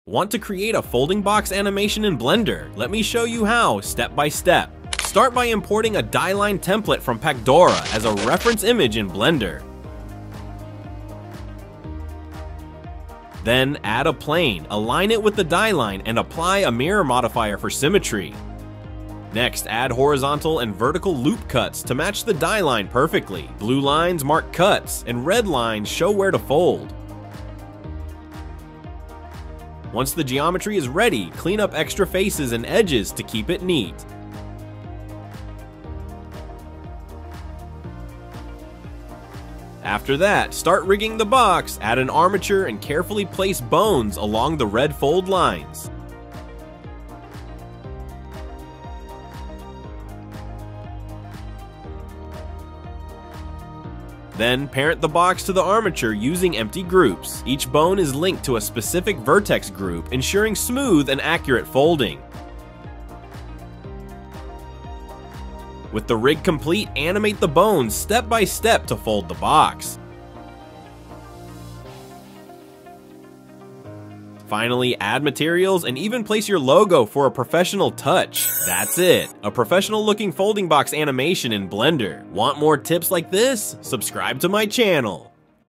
How to Animate a Folding Box in Blender–Quick Tutorial!
Learn how to create a stunning folding box animation in Blender in just 90 seconds! This quick tutorial is perfect for packaging design, product mockups, or adding animation to your portfolio!